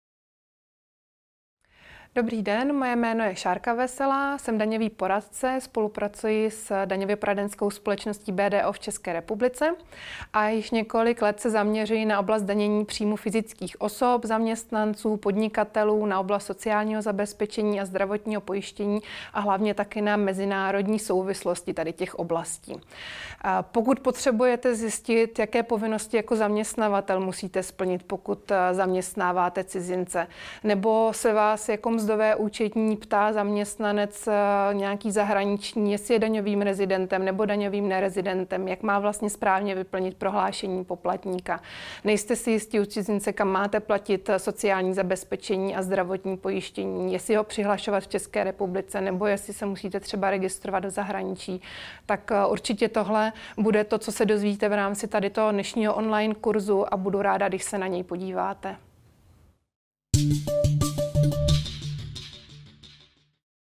Šárka Veselá: Specifika práce v zahraničí, daně a pojištění
Ukázkové video vzdělávacího kurzu.

Víte, jaké má český zaměstnavatel povinnosti při zaměstnání zahraničního zaměstnance v ČR? Umíte určit daňové rezidentství u zahraničního zaměstnance pracujícího v ČR? Jaké příjmy cizinců jsou zdanitelné v ČR a jak zamezit dvojímu zdanění v ČR? Kam platit pojistné na sociální zabezpečení v rámci EU (vyslání, souběžně vykonávaná činnost, výjimky) a jaká je situace rodinných příslušníků? To vše a mnohem více se dozvíte ve velice zajímavém video semináři Ing. Šárky Veselé z BDO Tax a.s.